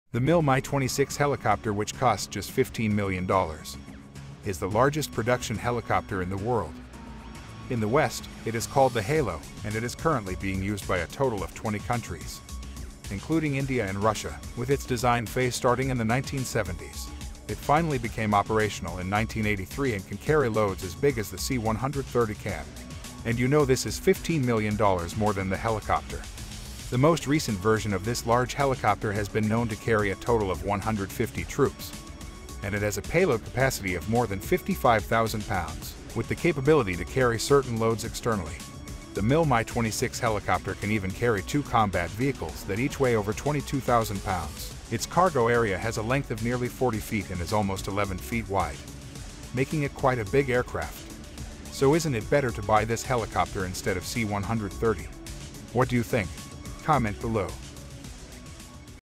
How many Military Trucks can Largest Helicopter Carry?
The Mil-Mi-26 helicopter  which costs just $15 million, is the largest production helicopter in the world.

  In the West, it is called the Halo, and it is currently being used by a total of 20 countries, including India and Russia.

With its design phase starting in the 1970s, it finally became operational in 1983 and can carry loads as big as the C 130 can.   and you know this is $15 million more then the helicopter

 The most recent version of this large helicopter has been known to carry a total of 150 troops, and it has a payload capacity of more than 55,000 lbs.

With the capability to carry certain loads externally, the Mil-Mi-26 helicopter can even carry two combat vehicles that each weigh over 22,000 lbs.

Its cargo area has a length of nearly 40 feet and is almost 11 feet wide, making it quite a big aircraft.

 so isn't it better to buy this helicopter instead of C 130?.  what do you think